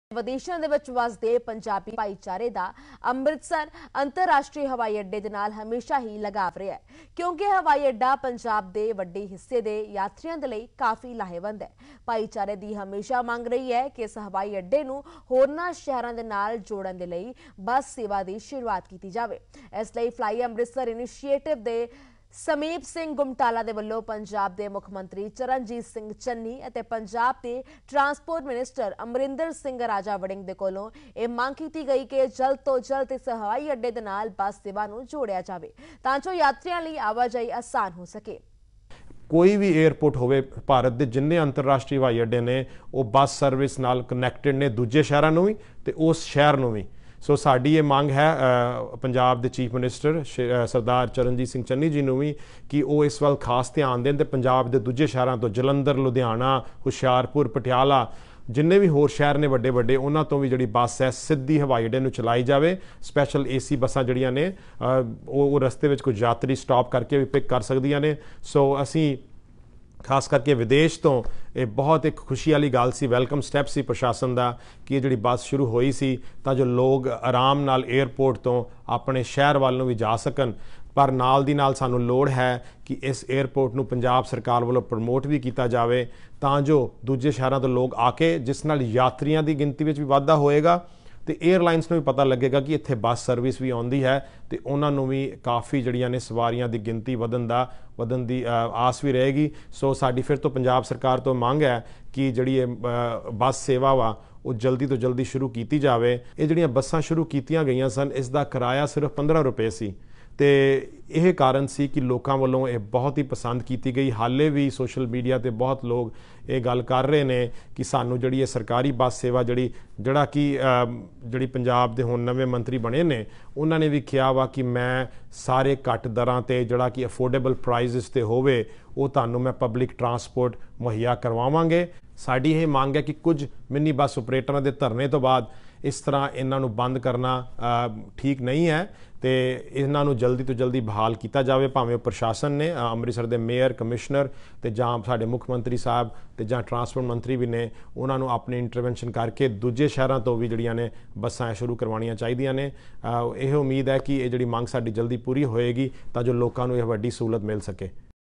NRIs Demand Bus Service For Amritsar Airport ਪਰਵਾਸੀ ਪੰਜਾਬੀਆਂ ਦੀ ਅੰਮ੍ਰਿਤਸਰ ਏਅਰਪੋਰਟ ਲਈ ਬੱਸ ਸੇਵਾ ਦੀ ਮੰਗ
Courtesy: PTC